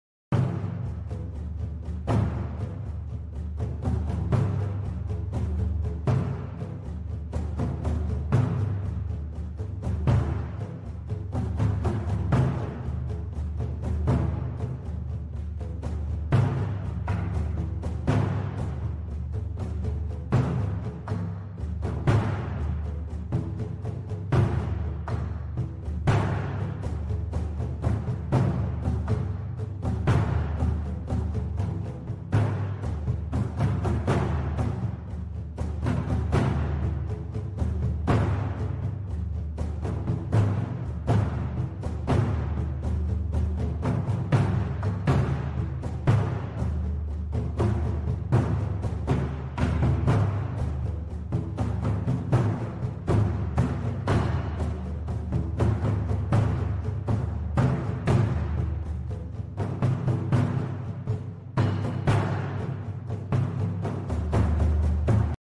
Person has caught spitting on consumer goods Coronavirus
Chinese has caught spitting on consumer goods Coronavirus. Intentionally trying to spread corona virus, disgusting act.